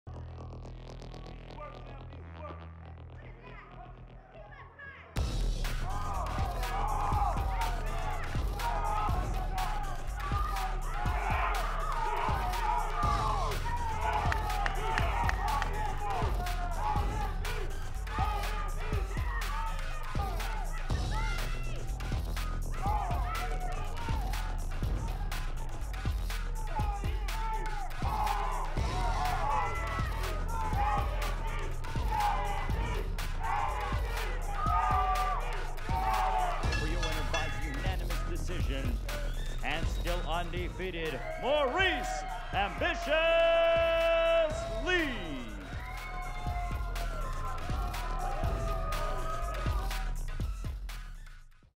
Maurice Lee vs Ronald Rivas Highlights 2.16.16
Mayweather Promotions boxer Maurice Lee took on Ronald Rivas at Sam's Town Live in Las Vegas.